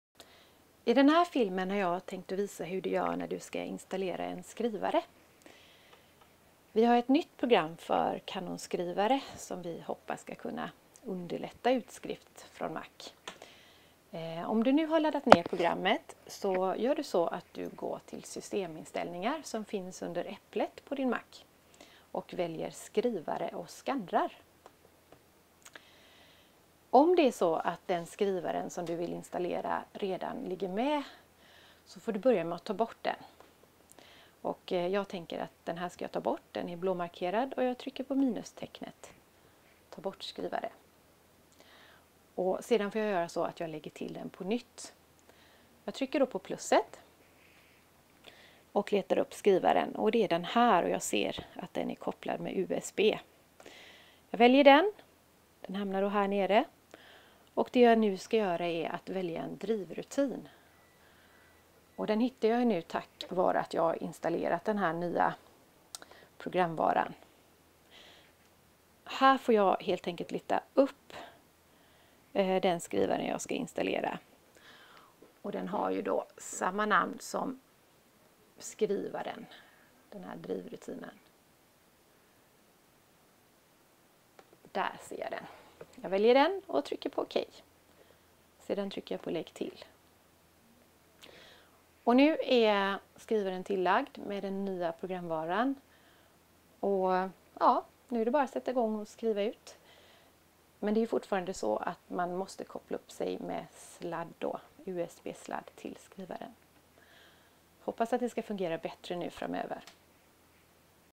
Installera Canon skrivare
Filmen är en guide om hur du går tillväga för att installerar Canon-skrivare på din Mac.